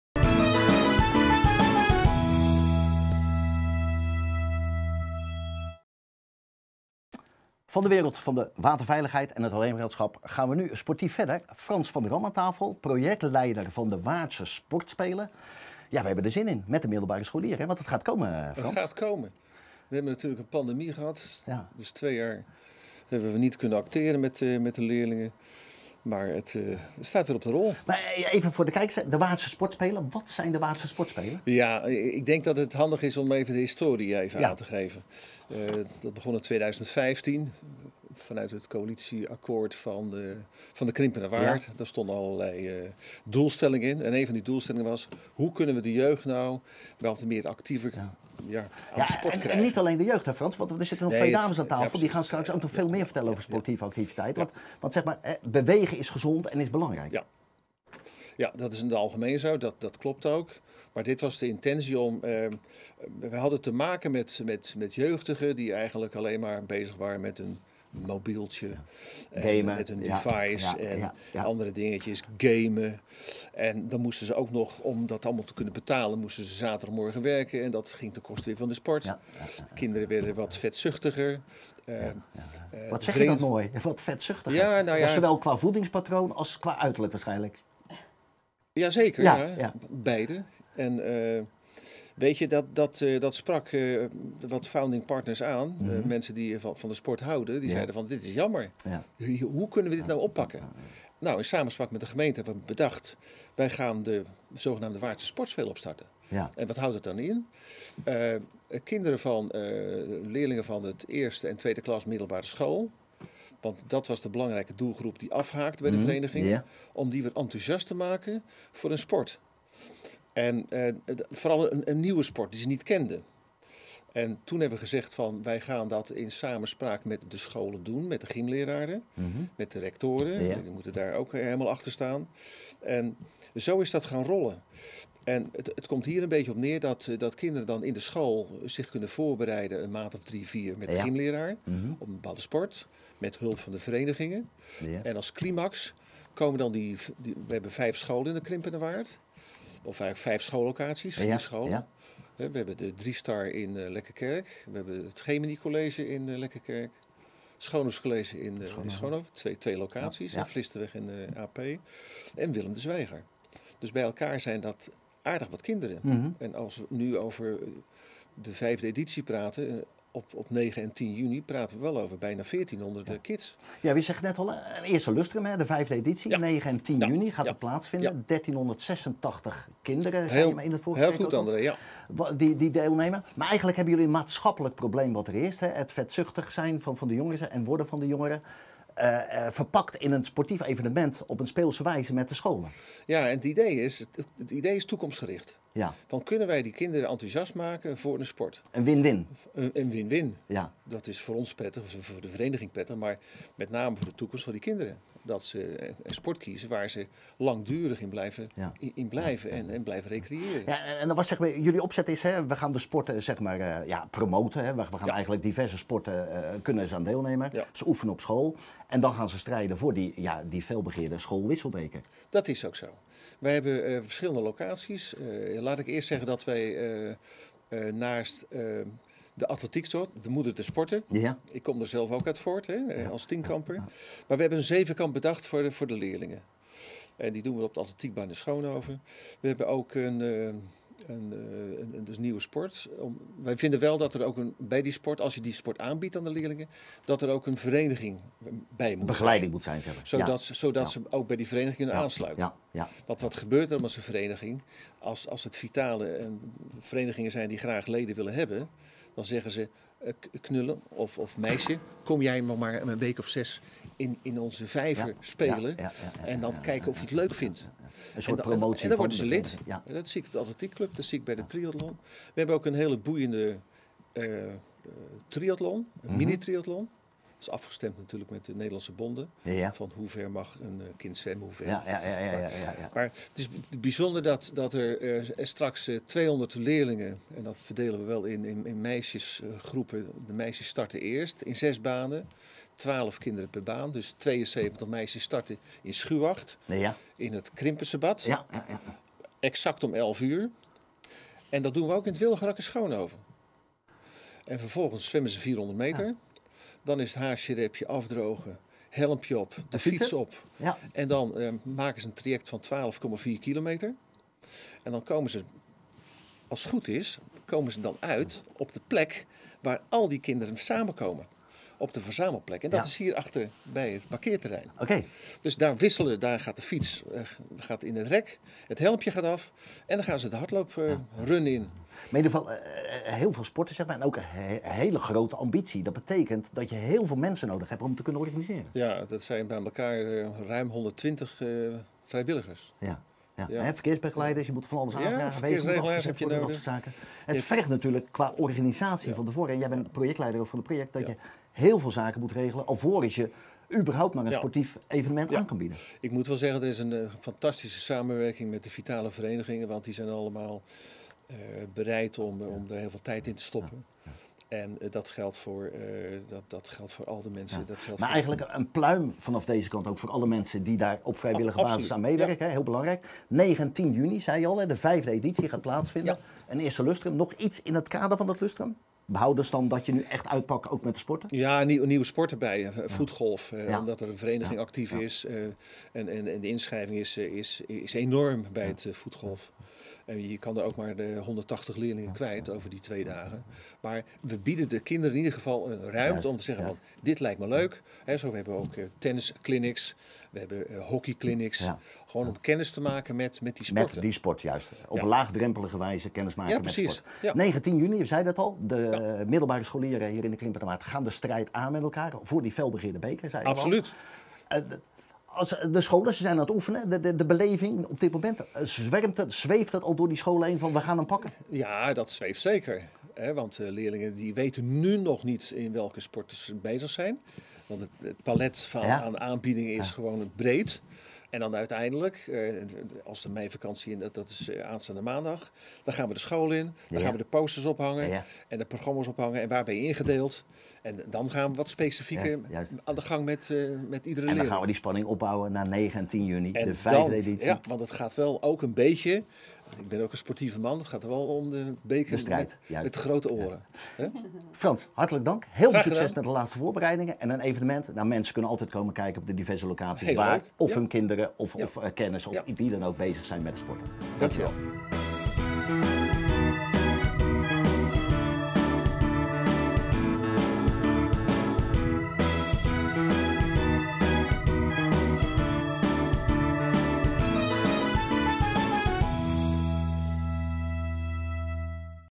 De Waardse Sportspelen
Bewegen en tegelijkertijd kennis maken met sporten dat is het motto van de Waardse Sportspelen. De projectleider Frans van der Ham neemt ons mee met deze sportieve happening voor de middelbare scholen in de Krimpenerwaard die dit jaar op 9 en 10 juni voor de 5e keer plaats vindt.